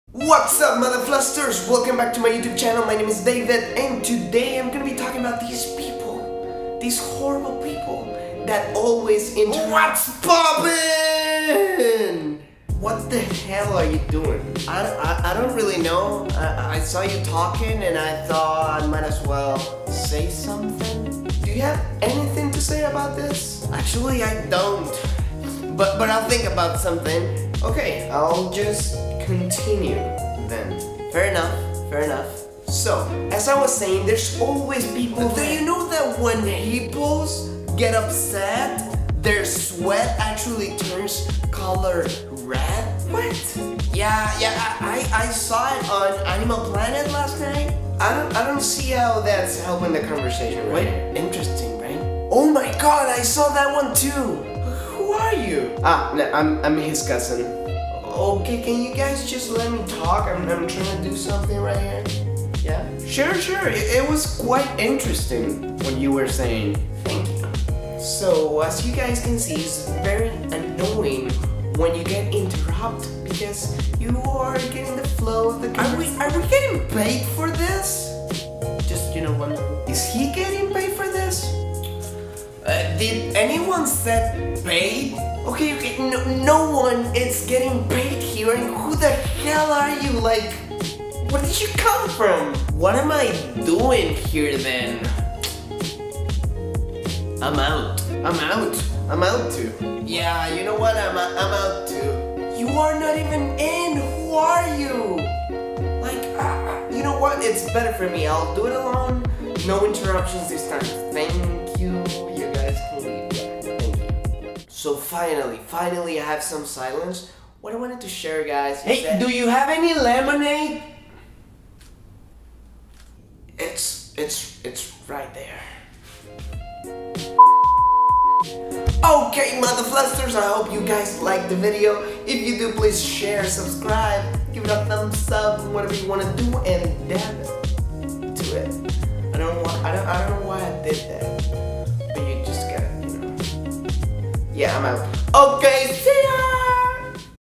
DEALING WITH INTERRUPTIONS
Have you ever been talking and people interrupts you? Worst feeling ever. Be polite, if someone is talking, let them finish. There is a right time for everything, even to talk about hippos! Hope you guys like the video. If you do, you know what to do.